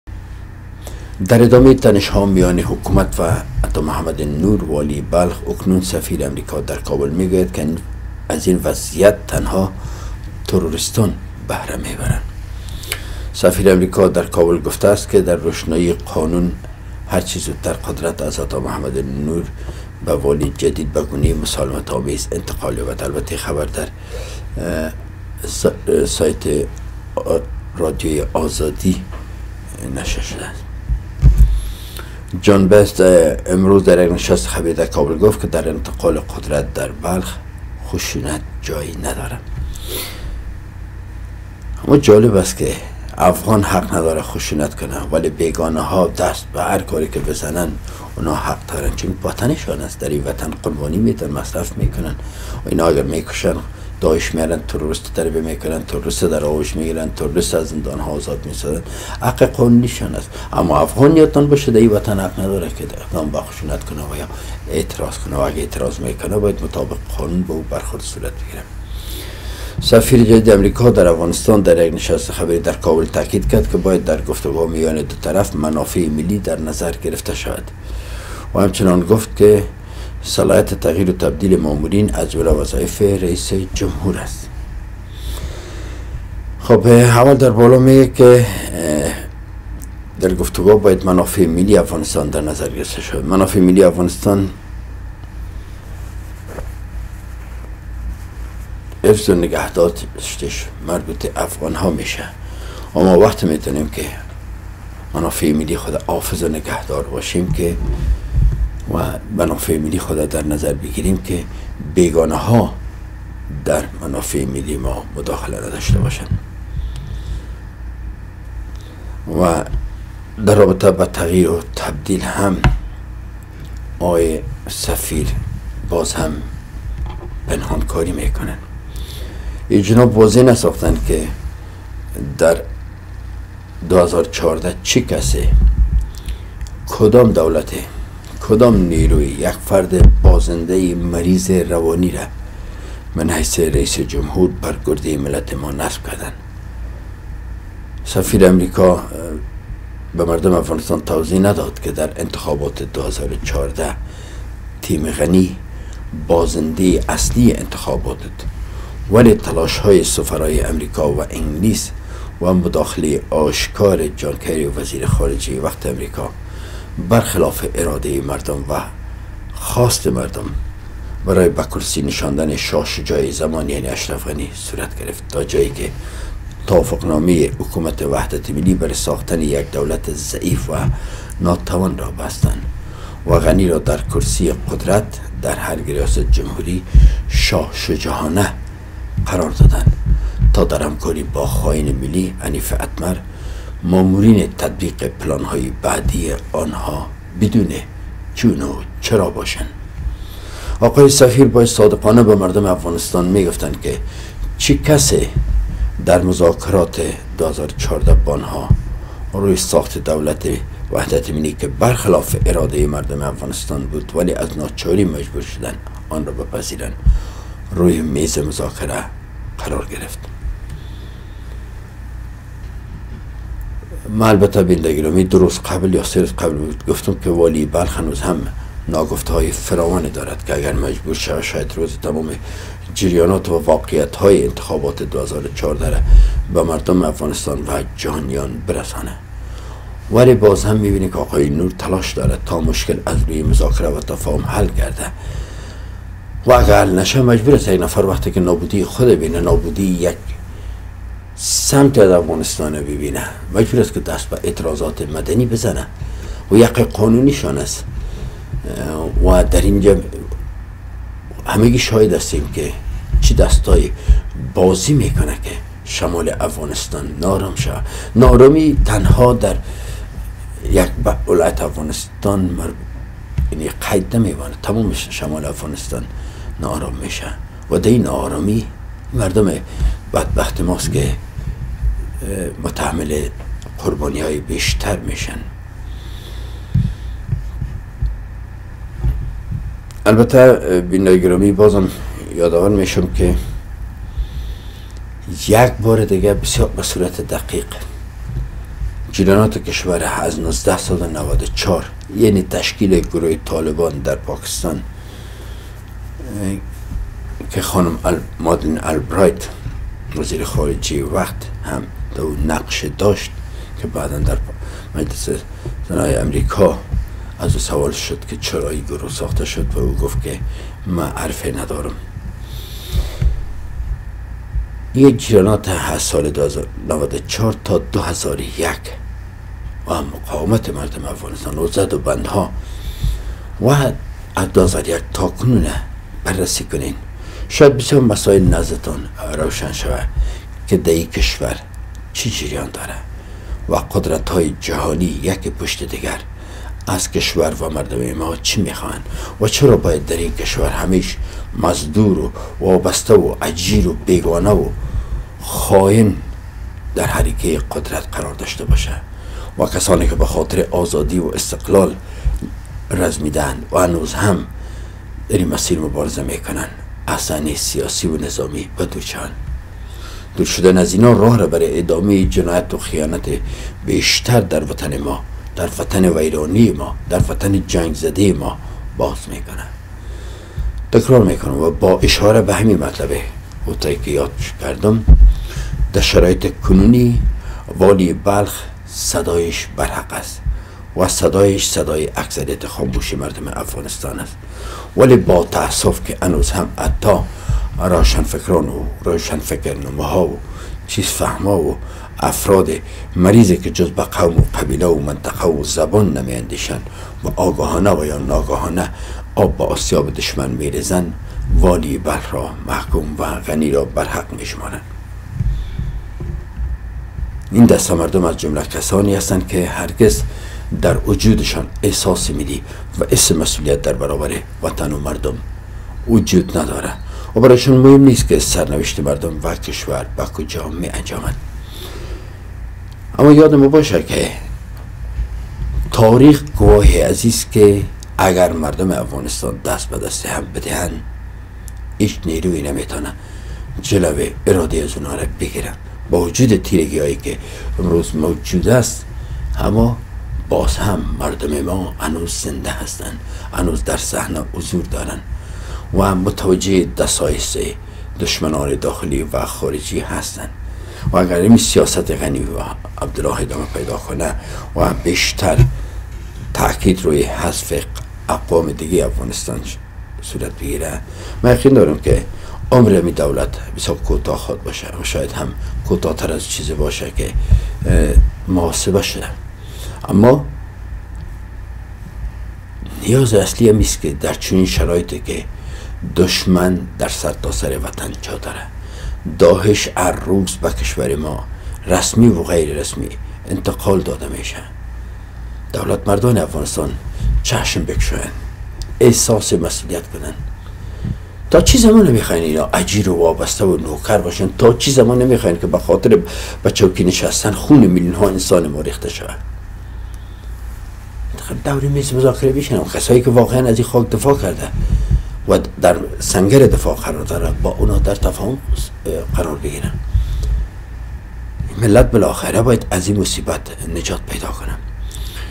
مکثی بر سخنان سفیر امریکا در کابل
سخنان سفیر امریکا در رابطه به بحران ارگ و بلخ مداخله آشکار در امور داخلی کشور ما است